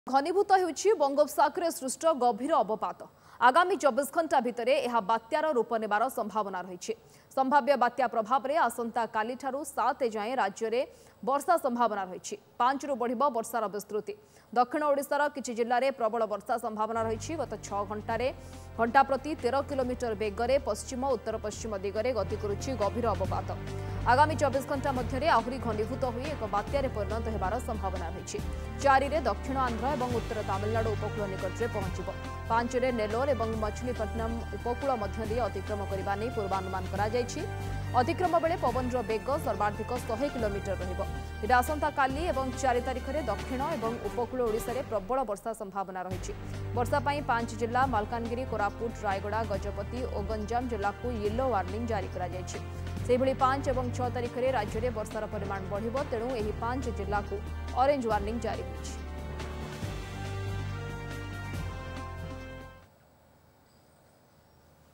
Cyclone formation in next 24 hours; Yellow warning issued for 5 Odisha districts
Cyclone Formation In next 24 hours; yellow warning issue to 5 Odisha districts